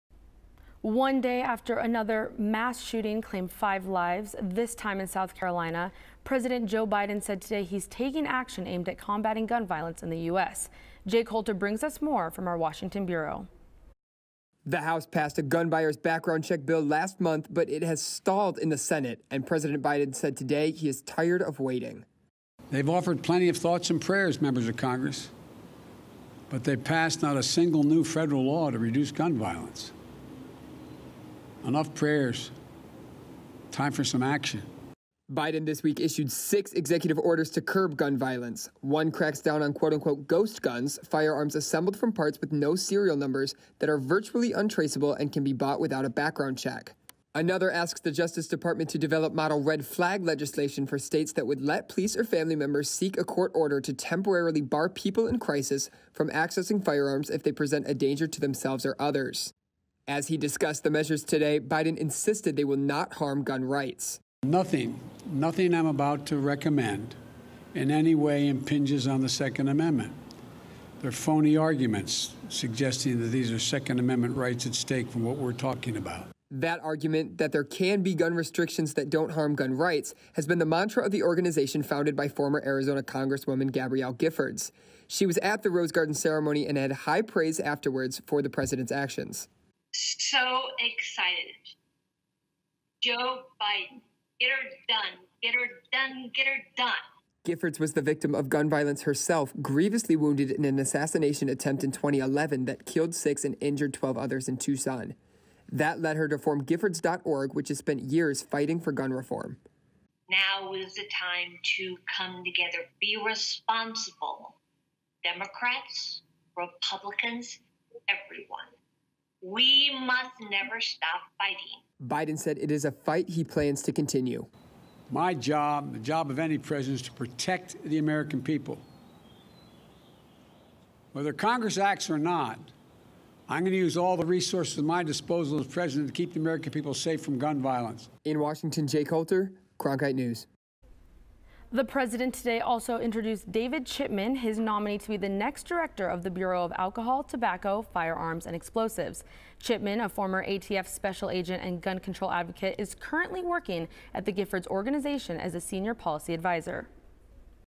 Gifford Guns | Cronkite News
As the White House unveils its plans for gun control, former Arizona Rep. Gabrielle Giffords joins a forum on efforts to "protect children physically and emotionally from the epidemic of gun violence."

(Video by Jake Holter/Cronkite News)